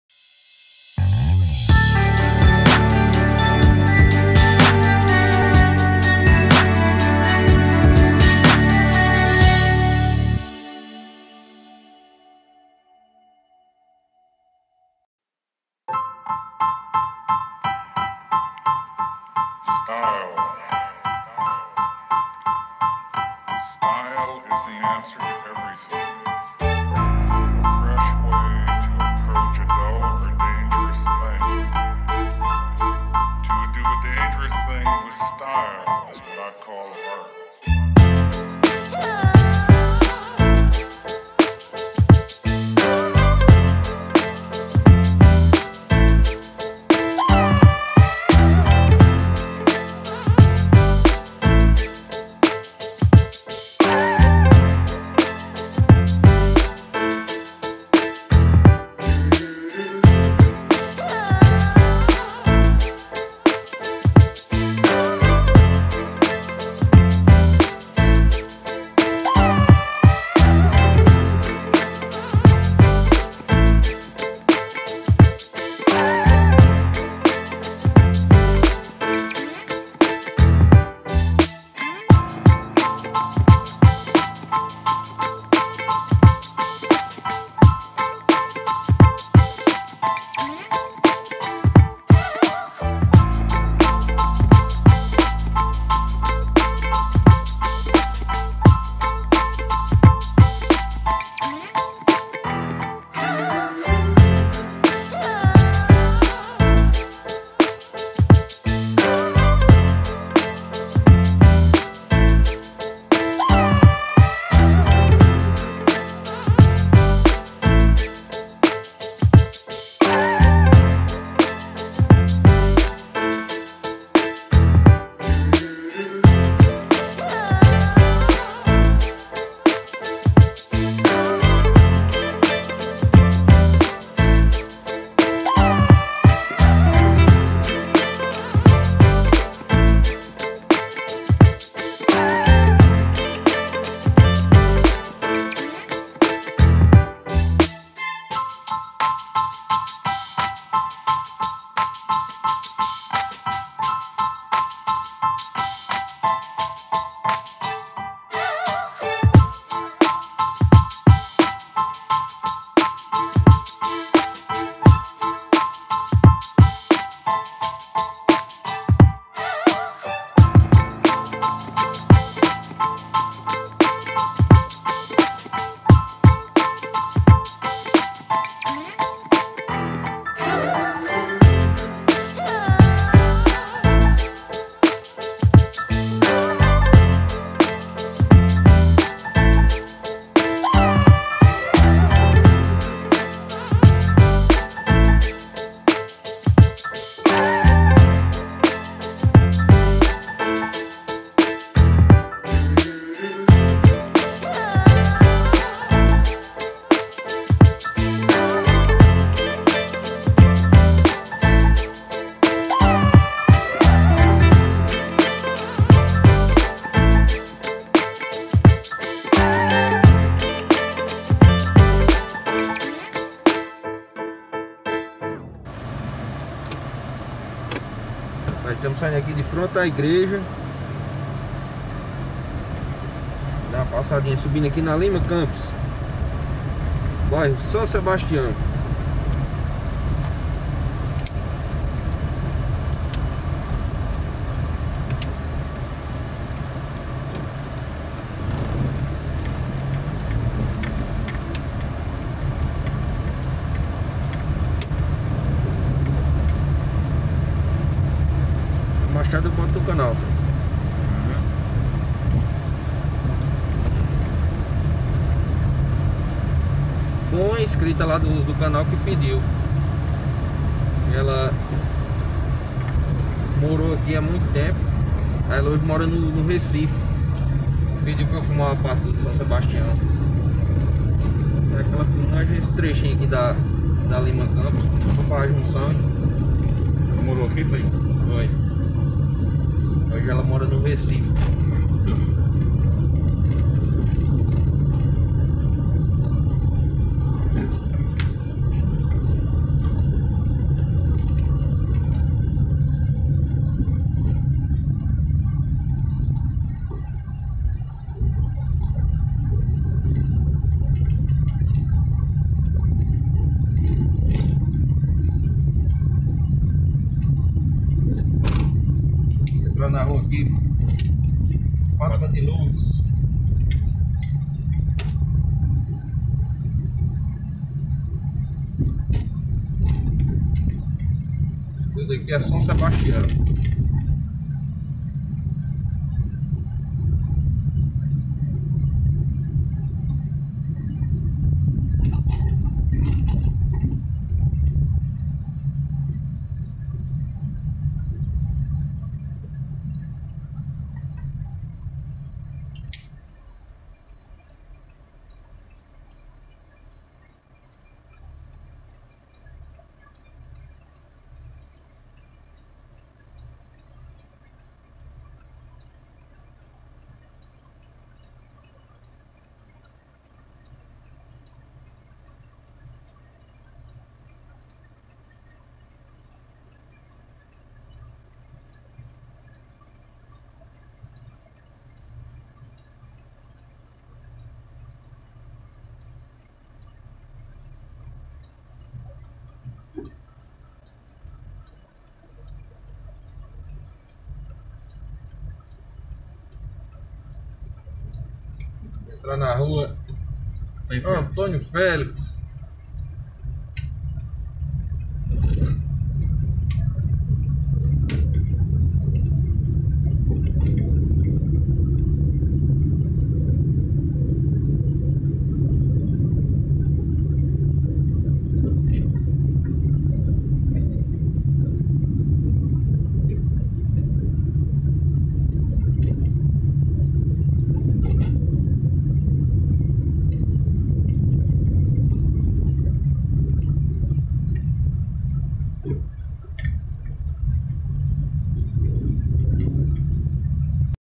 IGREJA CATÓLICA DE SÃO SEBASTIÃO
Um pouco da vista do bairro do são Sebastião em Patos PB.
um pedido de um inscrito do canal.